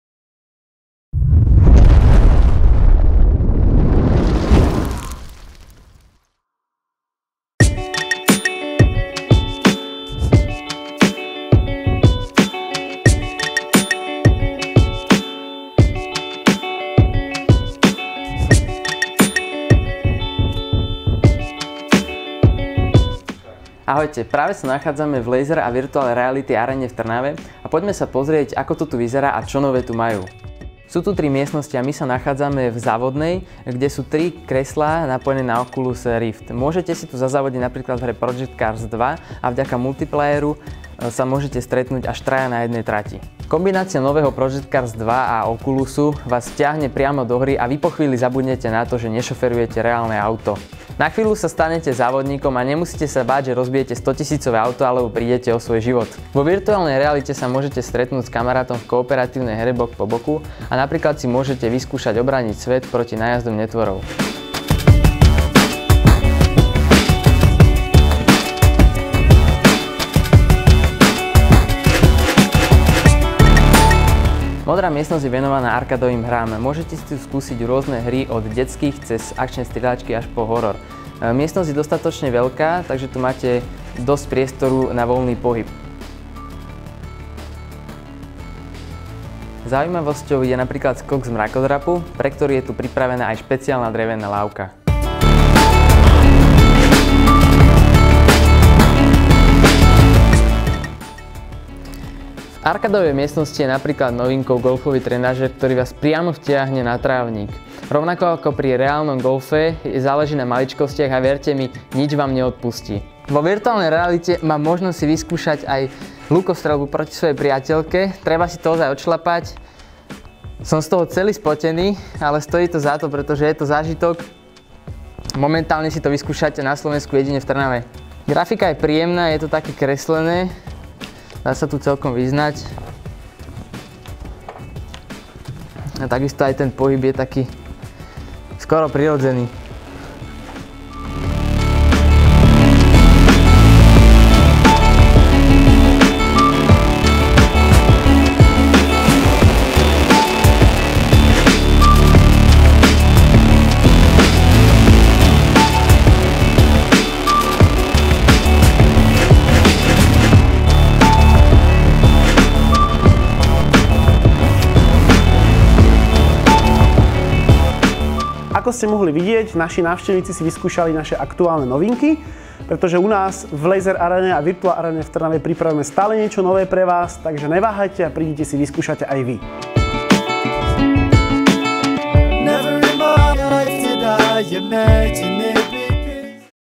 Laser Aréna, novinky 4K
Adam tentokrát vyskúšal aktuálne novinky v Laser a Virtual Reality aréne v Trnave.